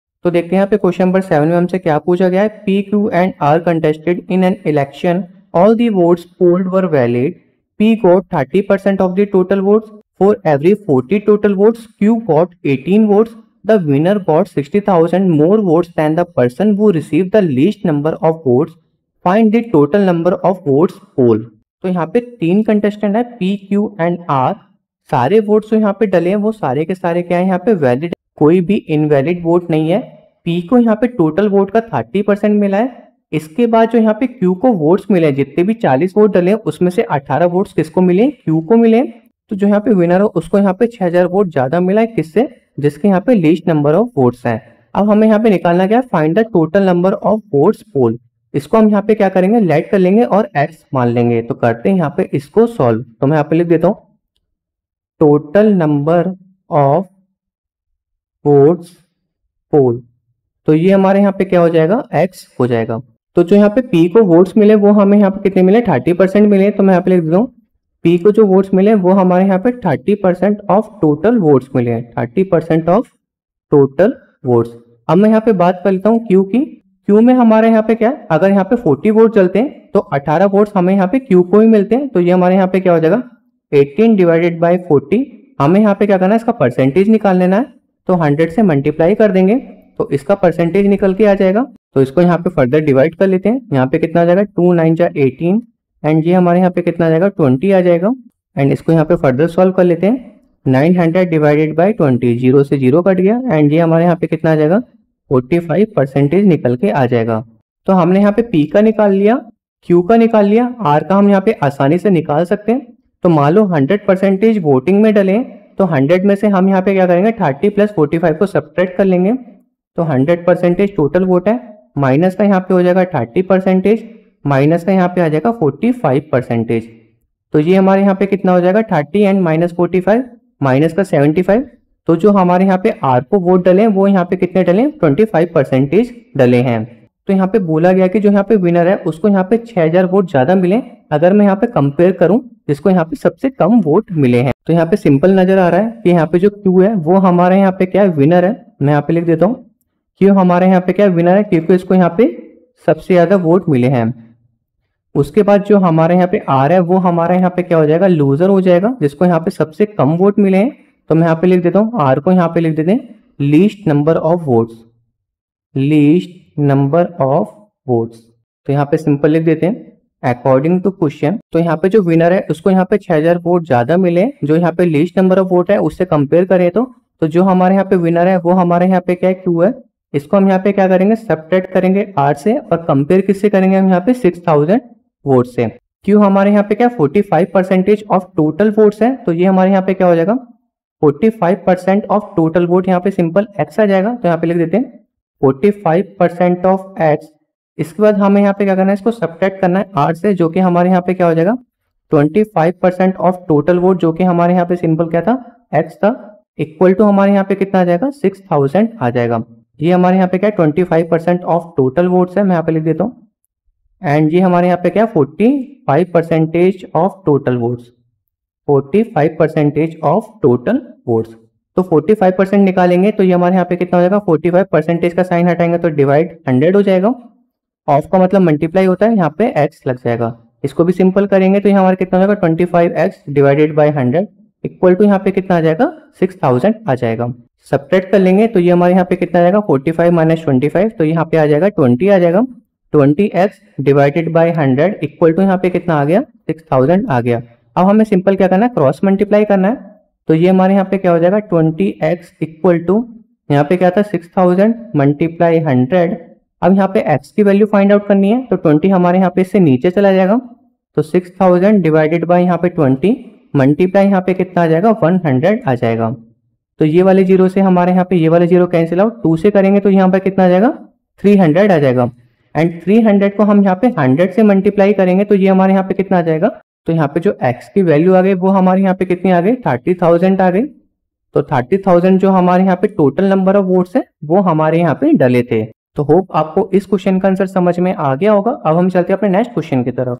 P Q and R contested in an election. All the votes polled were valid. P got 30% of the total votes.
Q.7 P, Q, and R contested in an election. All the votes polled were valid. P got 30% of the total votes. For every 40 total votes, Q got 18 votes. The winner got 6000 more votes than the person who received the least number of votes. Find the total number of votes polled.